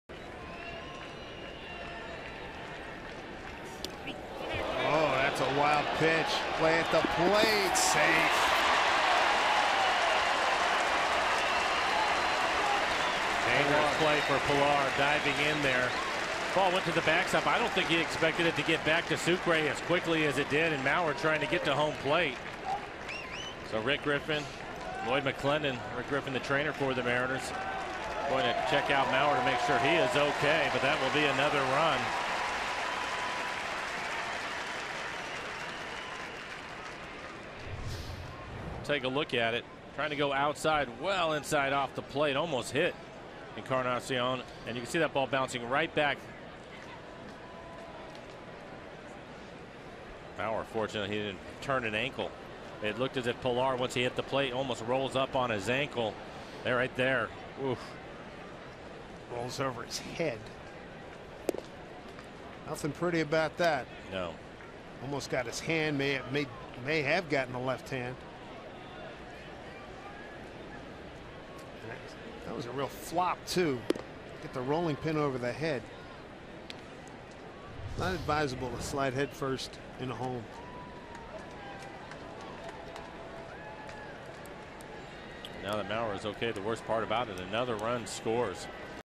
SEA@TOR: Maurer stays in game after plate collision
9/22/14: Brandon Maurer draws a visit from the trainer but stays in the game after Jose Reyes upends him while sliding into home